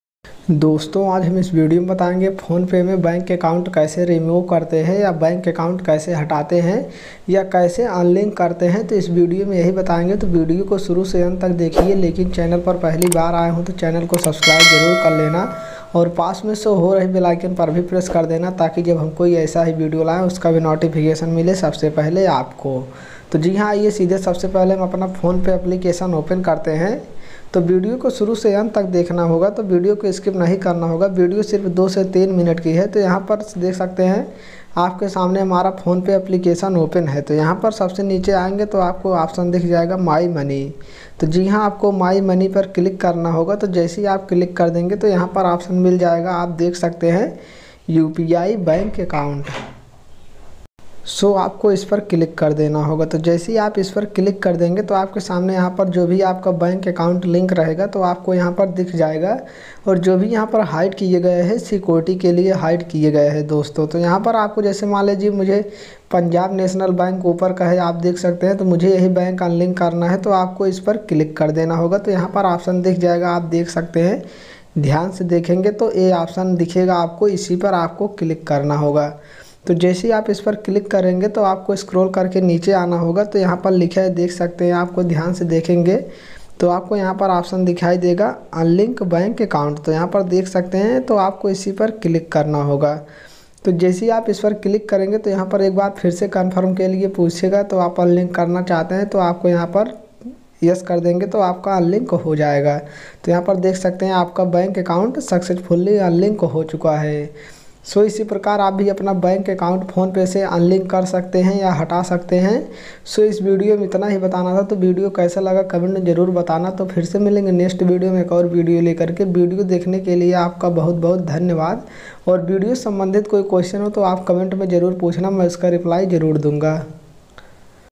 how to remove bank account from phonepe | how to delete bank account from phonepe | Atul Tech
Hi friends

नमस्कार दोस्तो स्वागत है आपका हमारे यूट्यूब चैनल में Atul Tech में
दोस्तों आज हम इस वीडियो में बताएंगे Phonepe से बैंक अकाउंट कैसे हटाते हैं या Phonepe में बैंक अकाउंट कैसे अनलिंक करते हैं तो इस वीडियो यही करके दिखाएंगे तो वीडियो को शुरू से अंत तक देखिए

और वीडियो से संबंधित कोई समस्या है तो आप कमेंट में पूछ सकते है

like shear comment

thanks for whaching